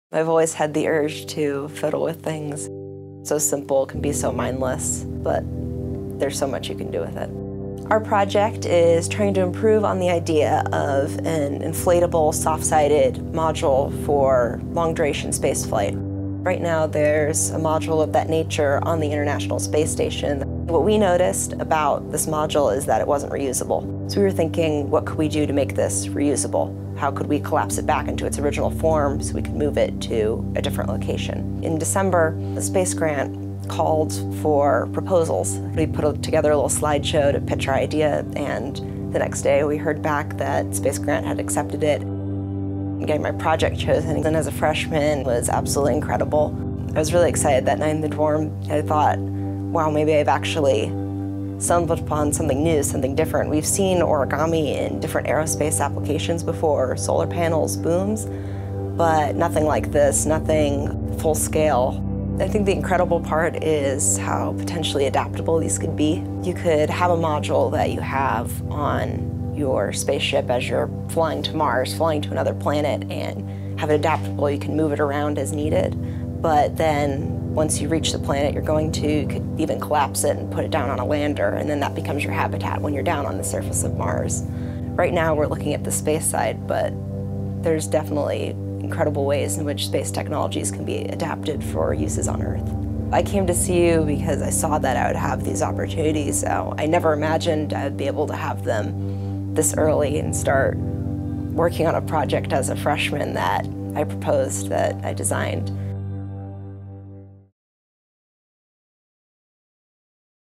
Lightbulb Moment: Origami and Aerospace
In an attempt to prototype a reusable crew module for spaceflight, undergraduates Anastasia Muszynski, Dawson Beatty and Andrew Pfefer found inspiration in origami and received support from the Undergraduate Research Opportunities Program (UROP).​